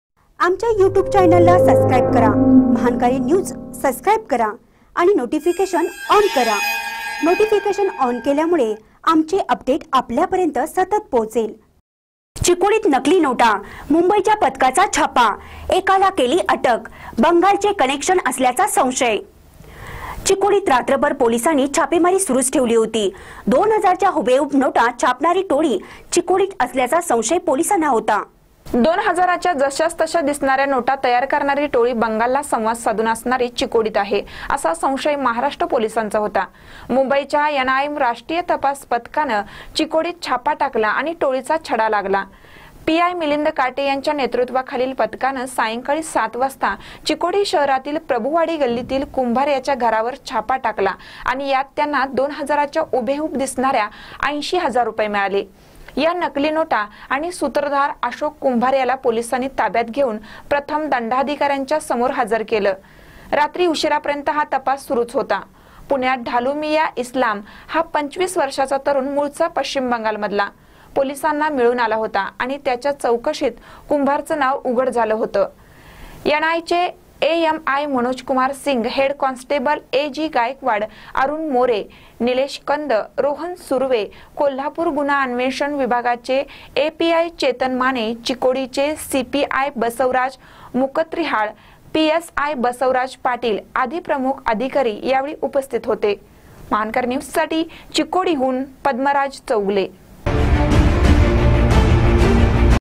चिकोडीत नकली नोटा. मुंबईच्या पथकाचा छापा. एकाला केली अटक
चिकोडीत नकली नोटा. मुंबईच्या पथकाचा छापा. एकाला केली अटक. बंगालचे कनेक्शन असल्याचा संशय